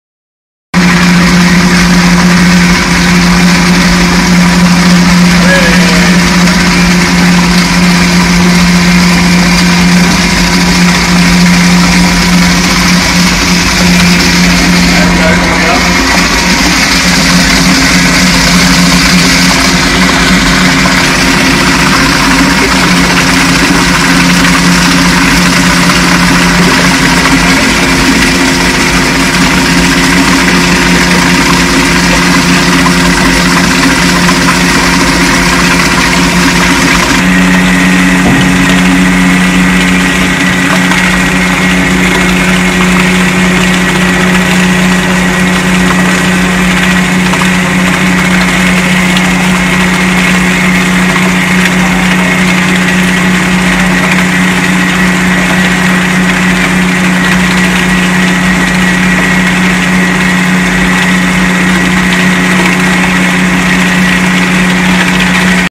Púlper Escuela del Papel de Tolosa
Desintegración de la pasta en el púlper al 12% de consistencia.